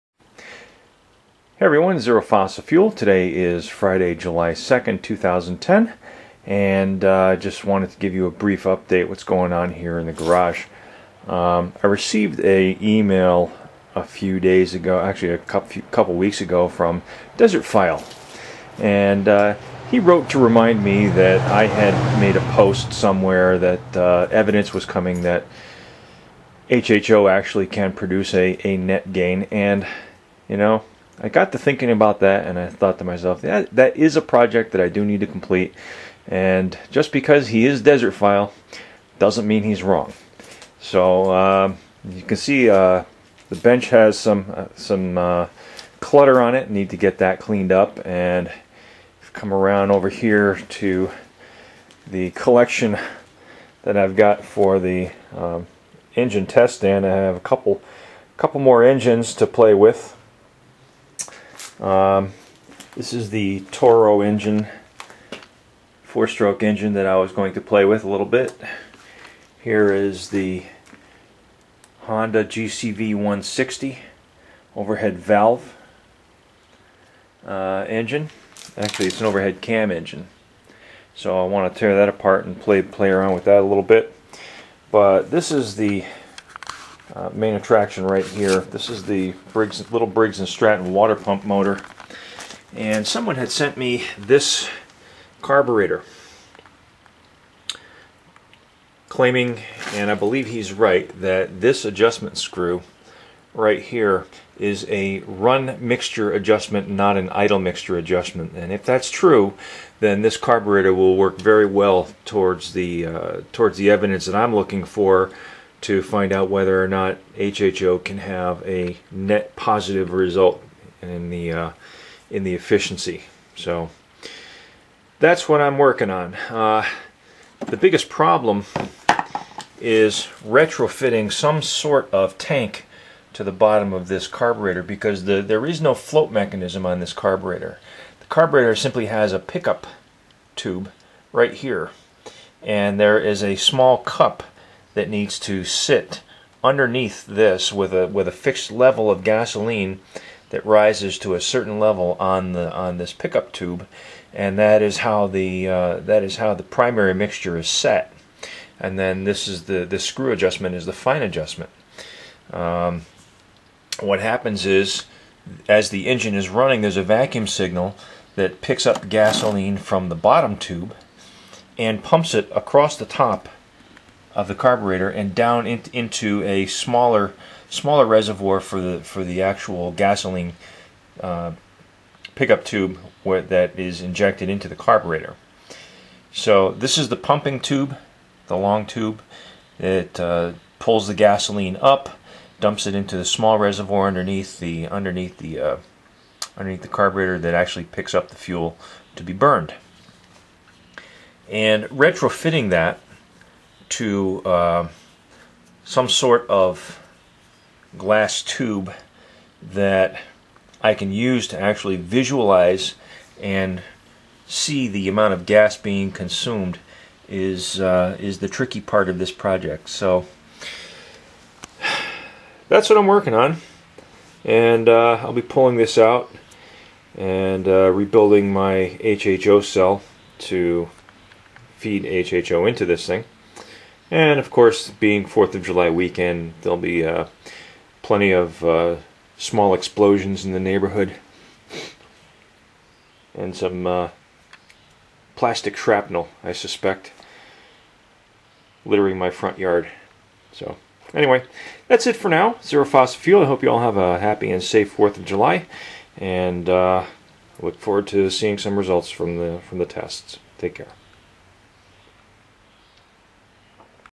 #230 - HHO run time experiment resumed
Desertphile reminded me I need to complete this project. Finally had the time and the ambition at the same time to resurrect the project and hopefully bring it to a satisfactory conclusion. Even I do not know for sure what direction it will lead me. If it works I will open it up to peer review. If it flops I will move on. Pretty simple.